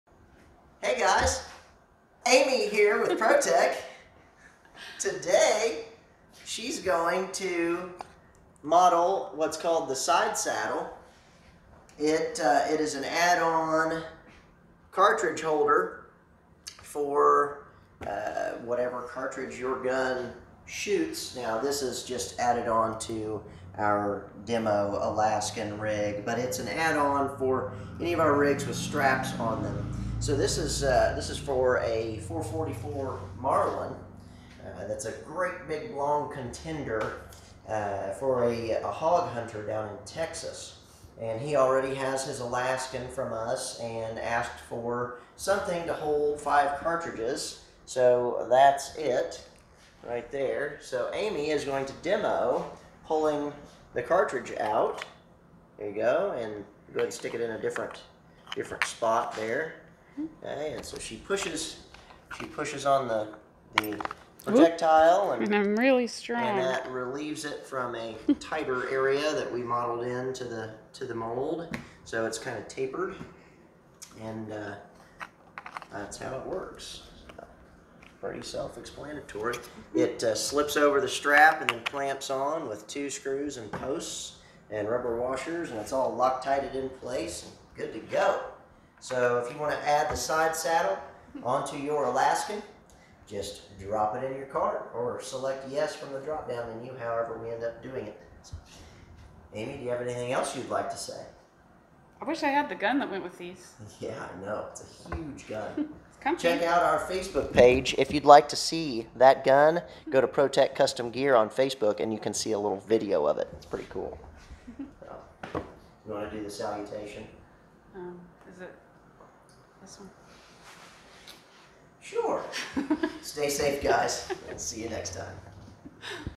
The SideSaddle. Cartridge or Magazine Holder for The Alaskan Chest Rig
Want to carry some extra ammo on your Alaskan?  If so, add a Side Saddle to it!